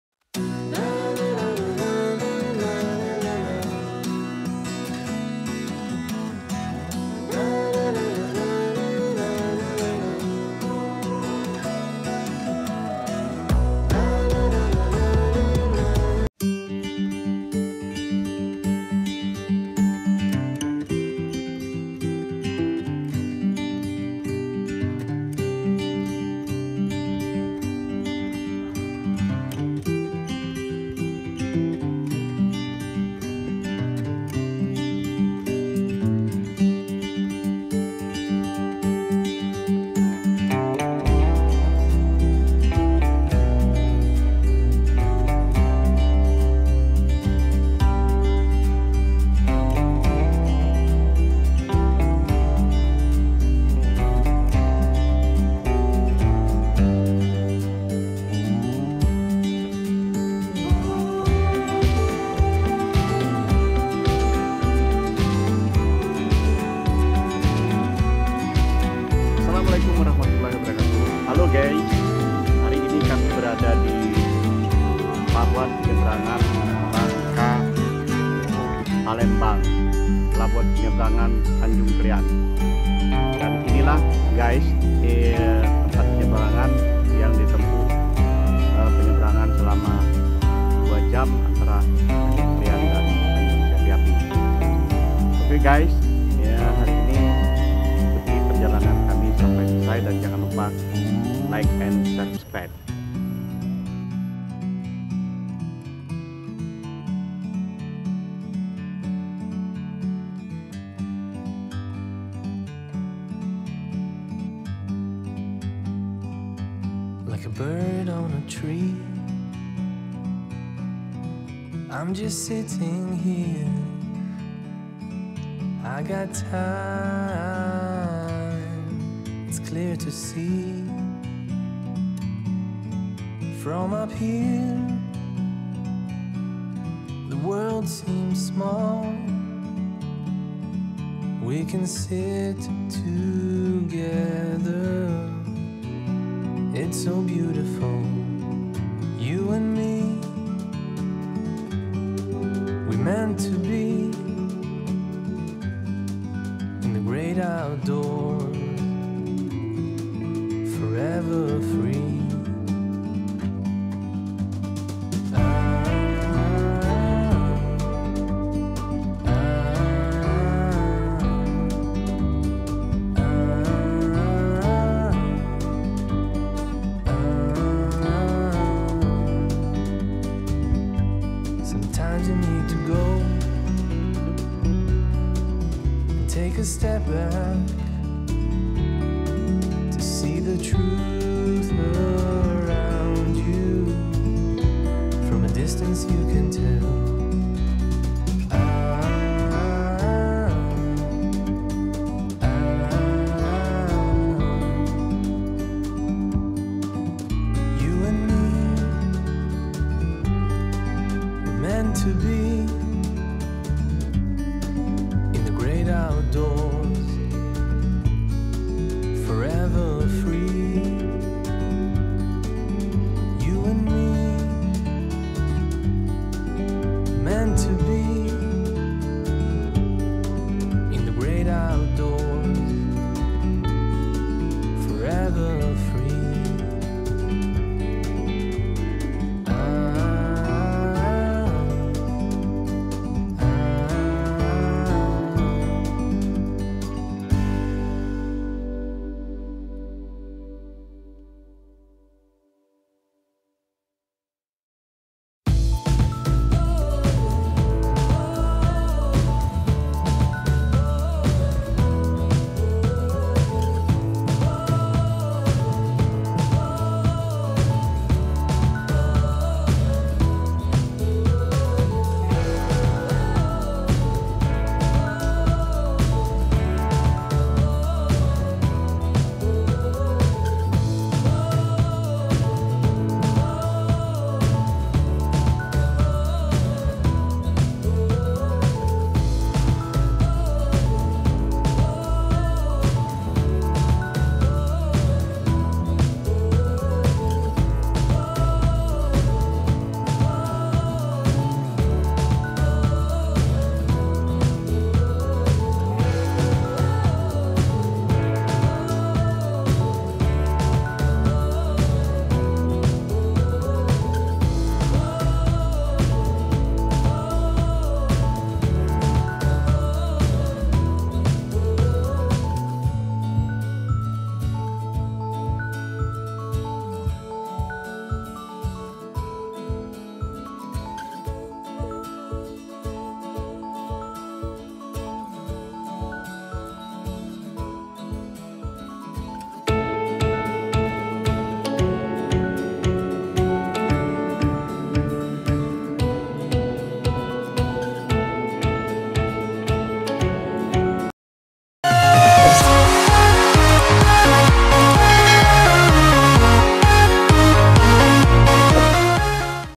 PELABUHAN TANJUNG KALIAN TERBERSIH DI INDONESIA.
Pelabuhan penyeberangan Bangka dan Palembang adalah pelabuhan Tanjung kalian yang berada di Muntok bangka barat, pelabuhan penyeberangan Tanjung kalian - Tanjung Api-api Sumatera,